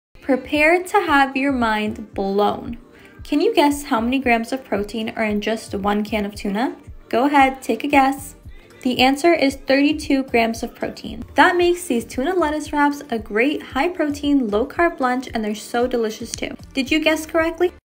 High Protein Tuna Salad Lettuce Wraps!
Tuna is SO slept on as a great source of protein! 🥲

Just one can of tuna has 32 g of protein… and add in the massive source of fibre from the lettuce and so many other nutrients 🤯

These tuna salad lettuce wraps are an absolutely amazing LOW carb HIGH protein lunch 🥬

Here’s how I make it:

1. Chop up 1 large red onion (or 1 small if you’re only using one can of tuna), along with a handful of pickles (chop in go small pieces) 🧅

2. Add your 2 cans of tuna to a bowl along with your onions and pickles, about 1 tbsp of light mayo (can also sub greek yogurt), about 1 tsp of mustard (I prefer Dijon but I didn’t have any), along with some black pepper.

(Tip: don’t add any salt if you’re using canned tuna like me, because even the ‘light’ tuna can be pretty salty; taste before you add salt)

3. Mix everything together well, and top with some chopped parsley as well.

4. Spoon your tuna salad into some lettuce leaves, wrap up and enjoy! 🤤

This has been one of my go-to recipes when I don’t know what to eat for lunch, and I hope it’ll be yours too! 🤩

It can be easy to resort to junk food when you don’t have anything meal prepped and ready to go when you’re hungry, but learning simple recipes like this that can be made in under 10 minutes are an absolute game changer! 🥰

Did you guess correctly? 👇🏻